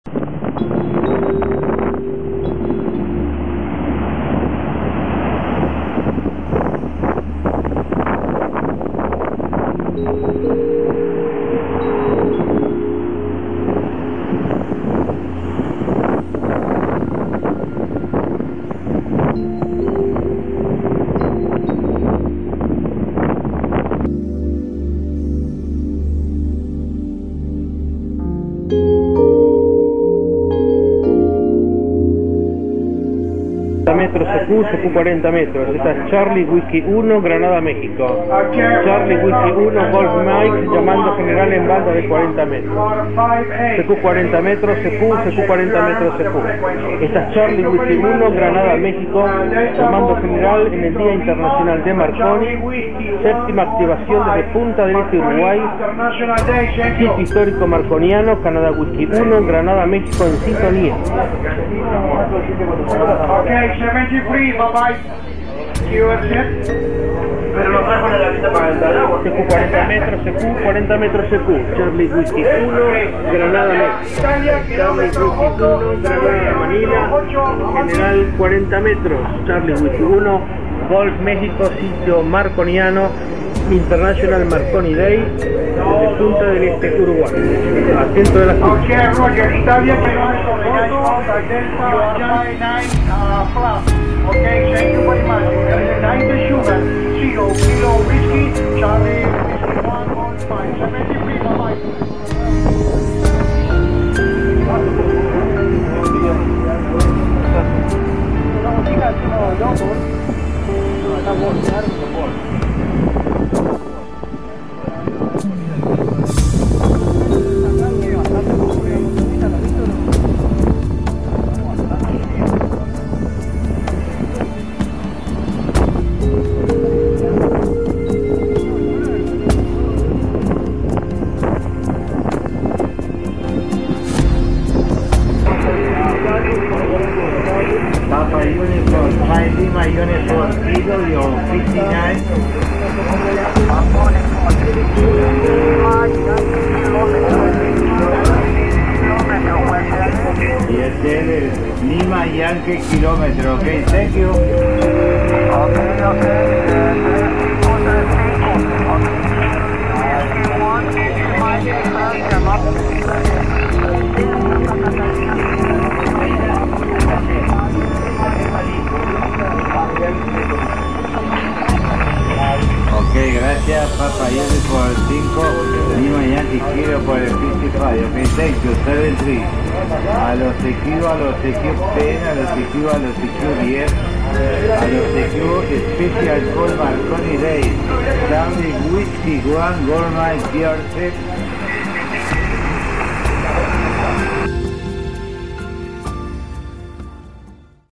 CW1GM. Punta del Este, Uruguay. 2012 International Marconi Day (Radiogrupo Sur).
7th consecutive activation of the historical radio telegraphy transmitter station site built by Guglielmo Marconi site with call MMO, (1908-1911). QTH: Punta del Este, Maldonado, Uruguay. Performed by members of Radiogrupo Sur, Uruguay, with the collaboration of  the Uruguayan Armed Forces. For the 25th edition of International Marconi Day, Apr 21, 2012.

7a activación consecutiva del sitio histórico de la estación de radiotelegrafía de Guglielmo Marconi con indicativo MMO, (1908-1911). QTH: Punta del Este, Maldonado, Uruguay. Por integrantes del Radiogrupo Sur, con la colaboración del Ejército Nacional de Uruguay y radioaficionados locales. Evento especial de la 25a. edición del International Marconi Day, Abr 21, 2012.